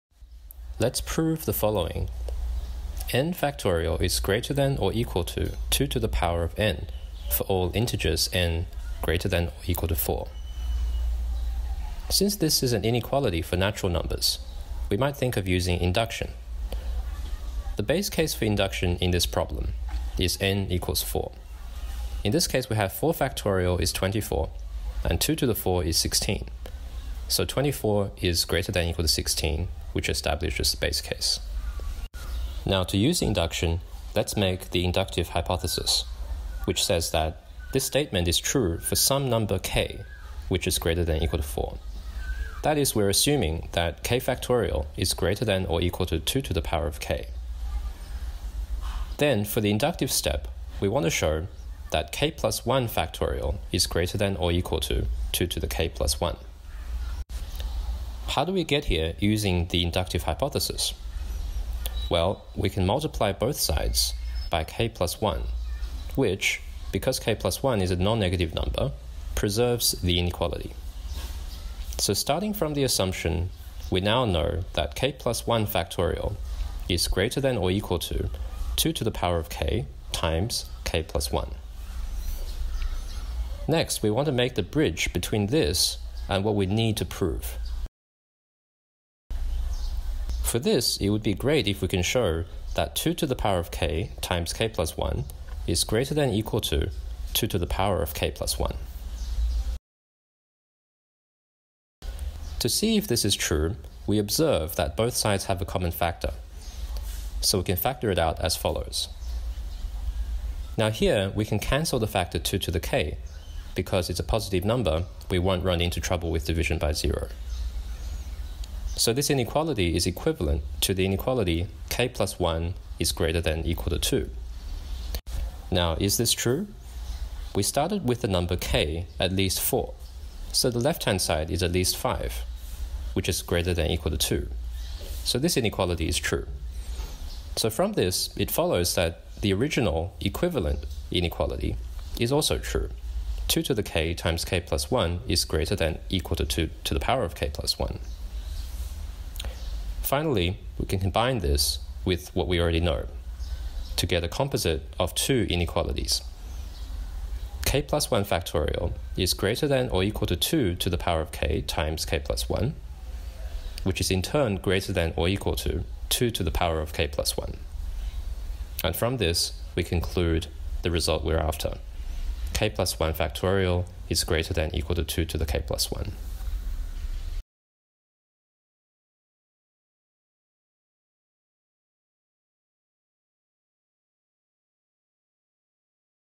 Factorial grows faster than exponential
A proof that the factorial function grows faster than the exponential function.